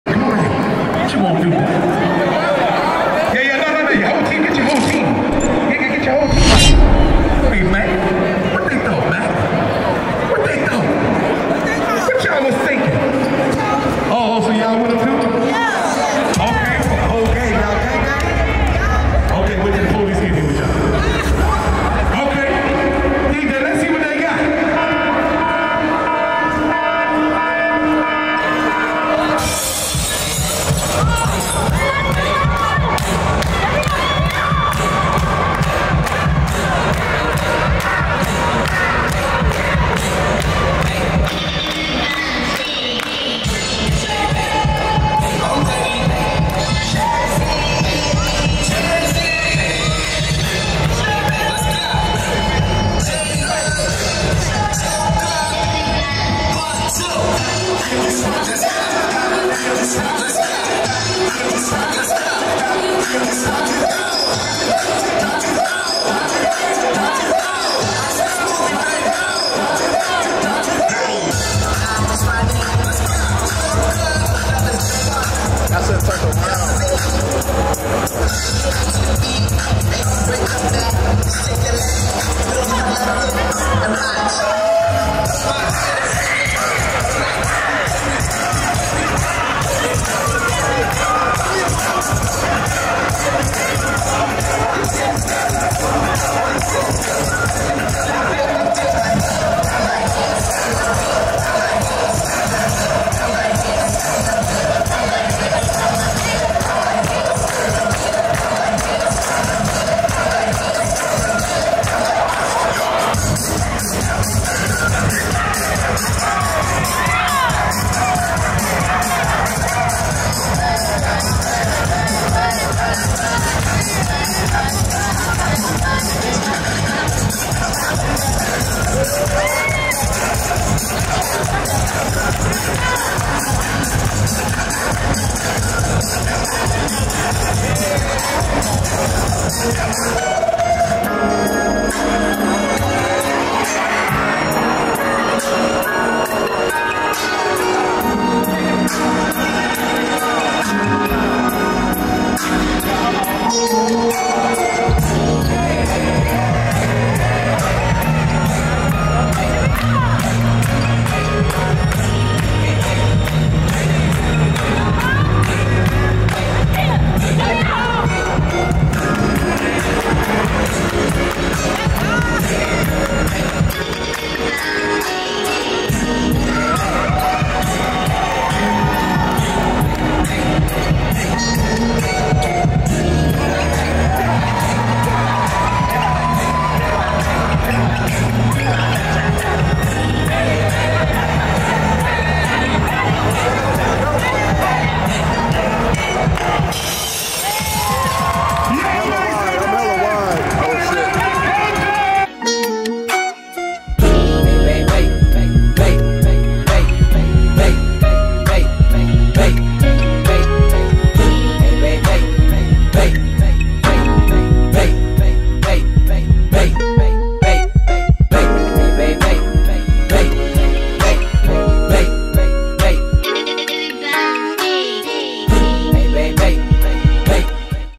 OBL ( ONES BASKETBALL LEAGUE ) 2022 IN BROOKYLN x REPTHECULTURE INTERMISSION PERFORMANCE
WE DID WHAT NEEDED TO BE DONE! THANK YOU TRACY MCGRADY & HOLLYHOOD BAYBAY FOR ALLOWING JERSEYCLUB TO BE PRESENTED ON A OBL BASKETBALL PLATFORM !!  WHAT A MOMENT THIS WAS FOR THE JERSEYCLUB CULTURE AS A REP THE CULTURE COLLECTIVE‼️
DANCERS : @QUEENN_SHAYYY @KIA_BHN  @FAVV.NEYY @KU2FED @ESKILLLZ @_21_JUICE @TRXLLVOICEOFFICIAL @BIGSEXYYYY973_ ❤️👑
RECORDED BY : @SORANDOMVISION 🔥
FOLLOW @rep.theculture  FOR MORE DANCE VISUALS & FOR MORE JERSEY CULTURE CONTENT ‼️🔥